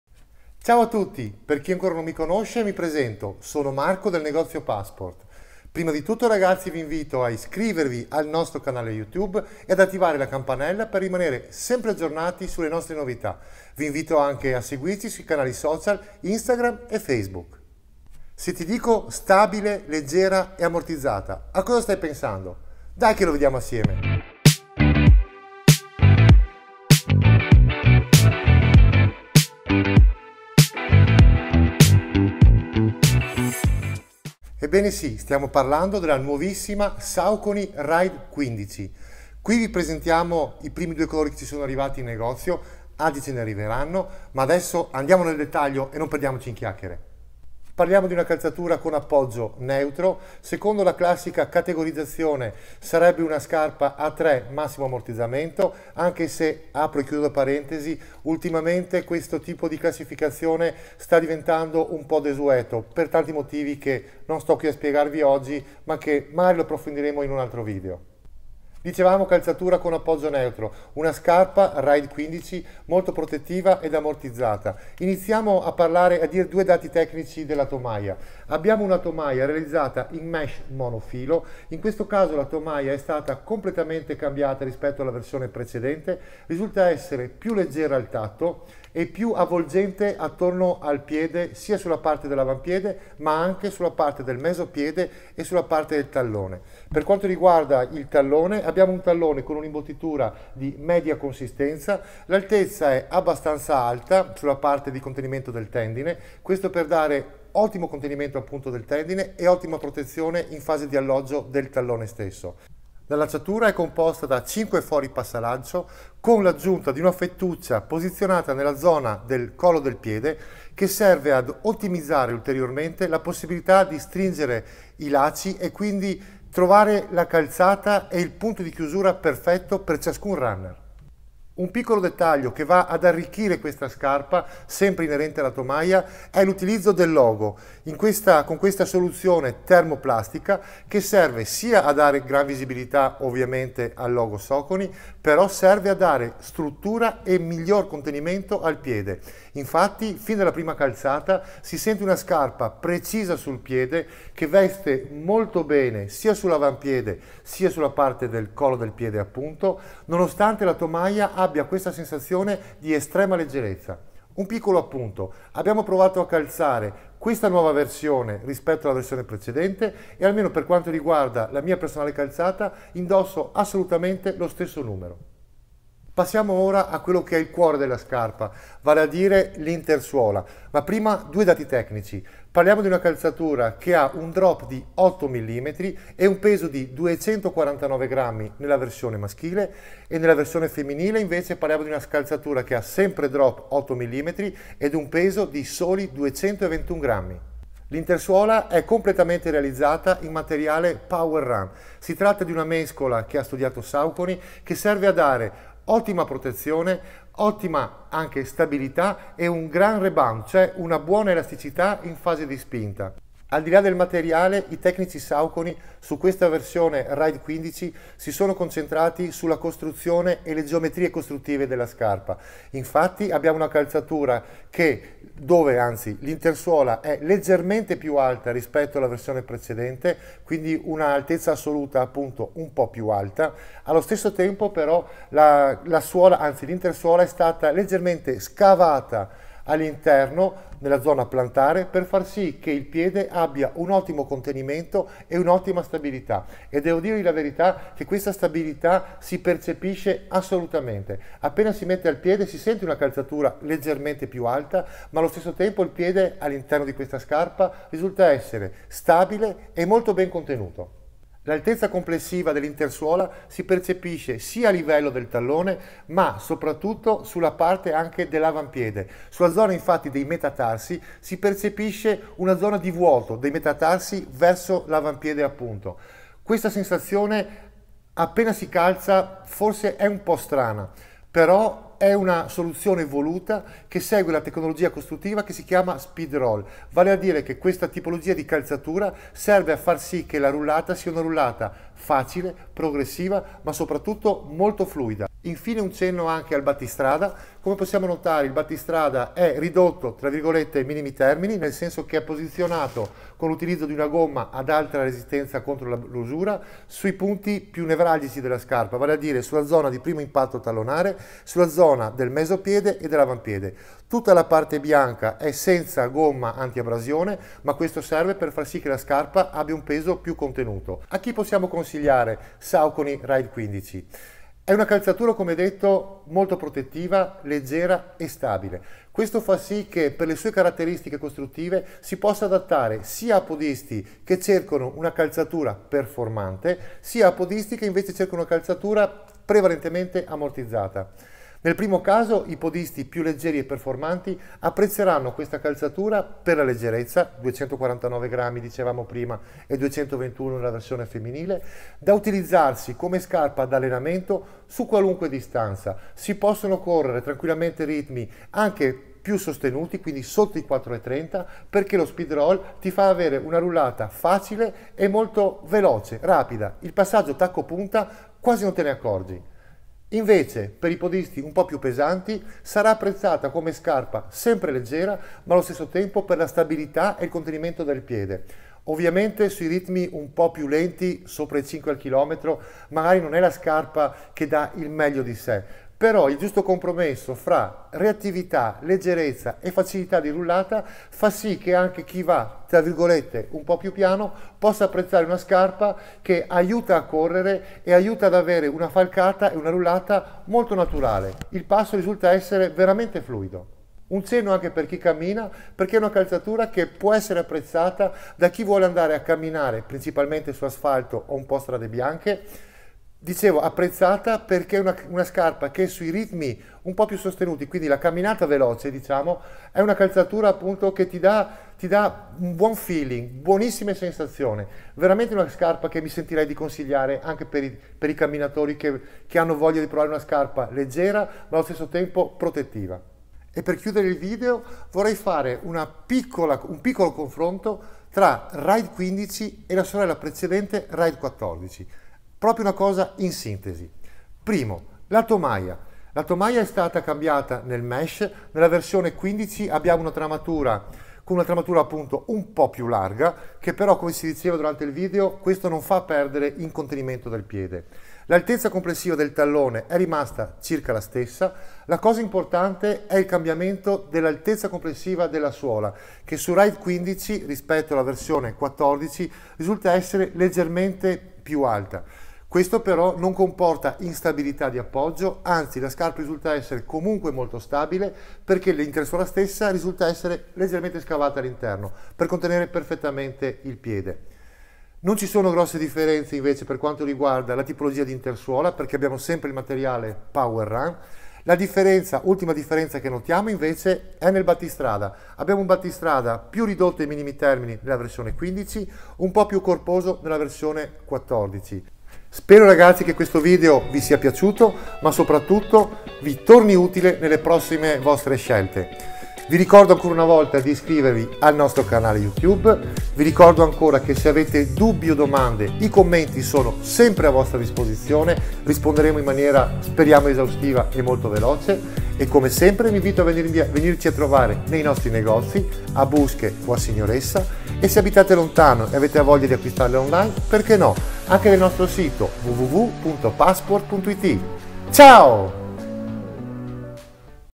Saucony Ride 15. Caratteristiche tecniche e prime impressioni
Saucony Ride 15. Leggera, protettiva e ammortizzata. Scopriamo le tante novità che Saucony Ride 15 propone a tutti i Runner che cercano la scarpa da running per allenamenti quotidiani su asfalto e strada bianca.

SINTESI VIDEO
00:24 Introduzione
01:18 Caratteristiche tomaia
03:02 Peso e Drop di Saucony Ride 15
03:18 Intersuola
04:55 Il Battistrada
05:21 Consigli di utilizzo
07:13 Confronto Ride 15 VS Ride 14